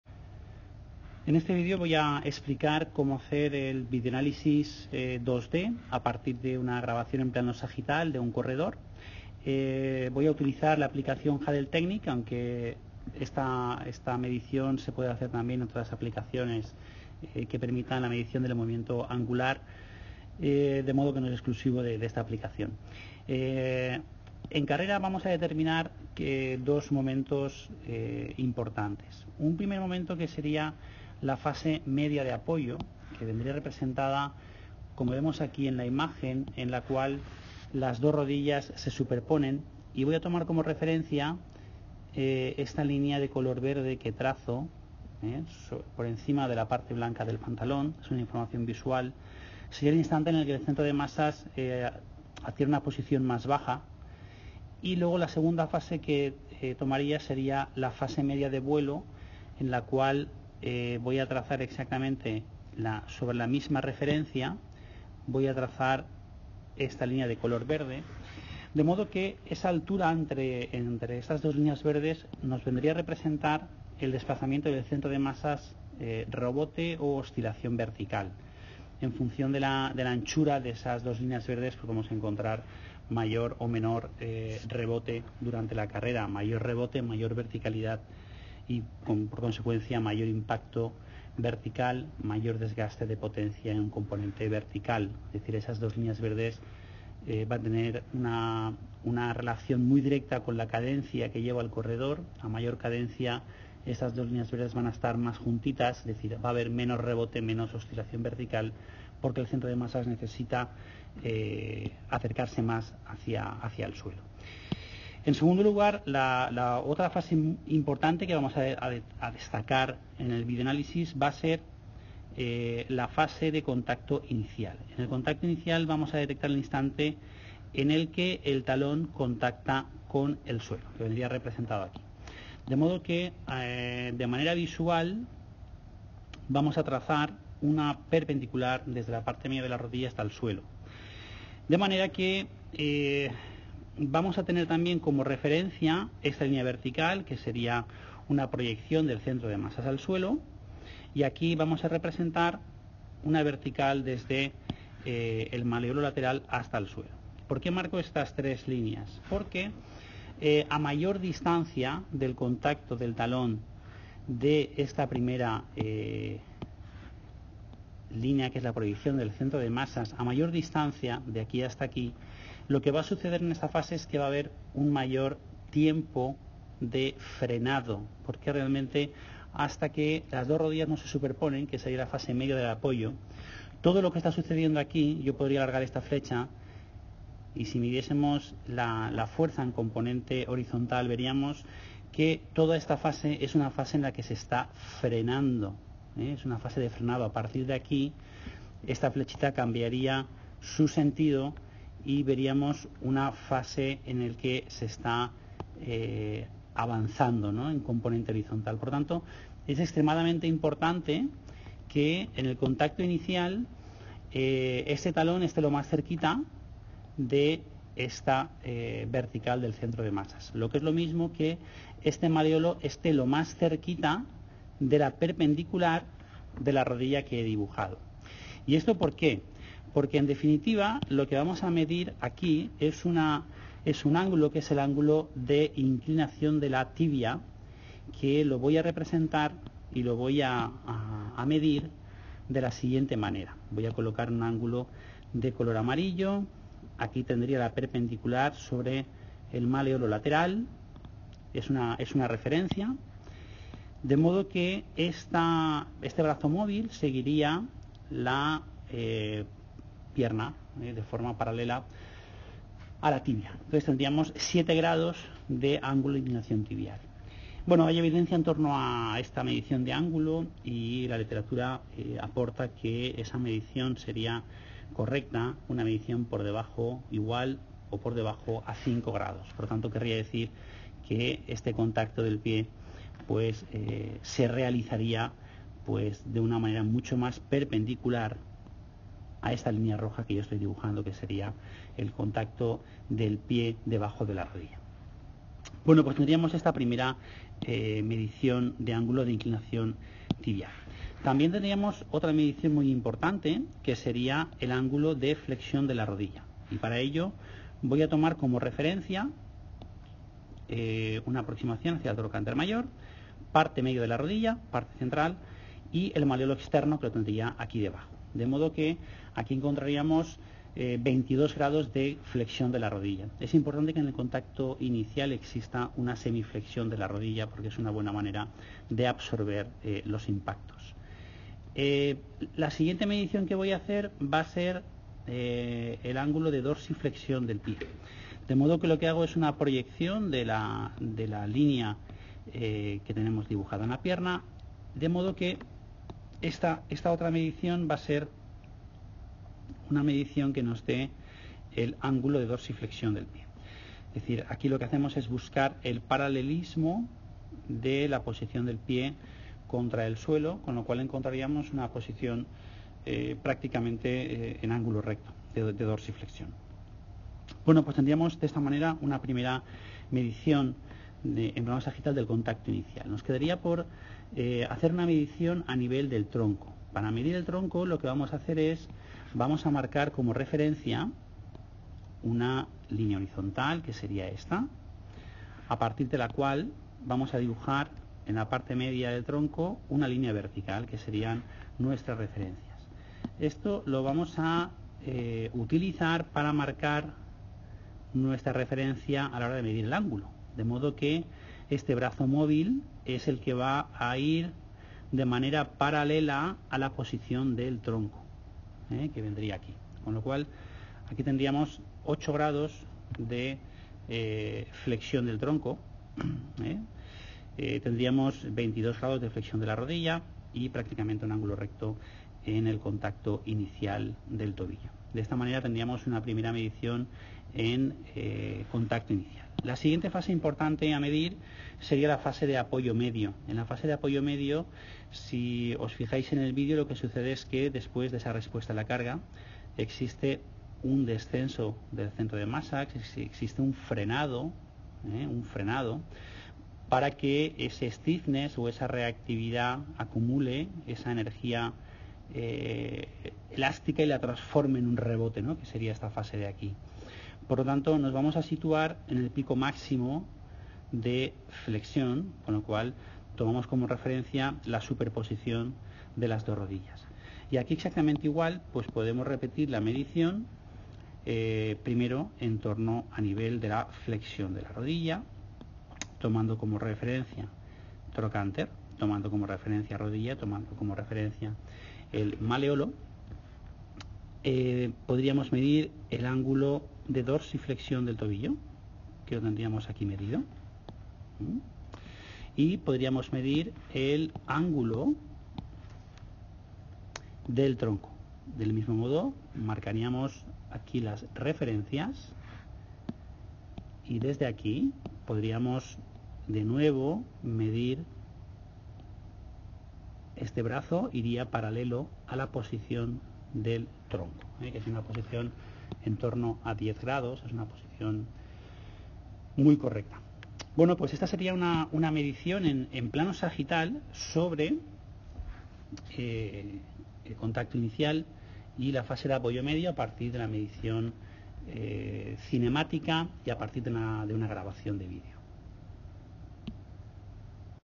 Vídeo análisis de carrera
Contenido: se explica cómo realizar el vídeo análisis 2D de carrera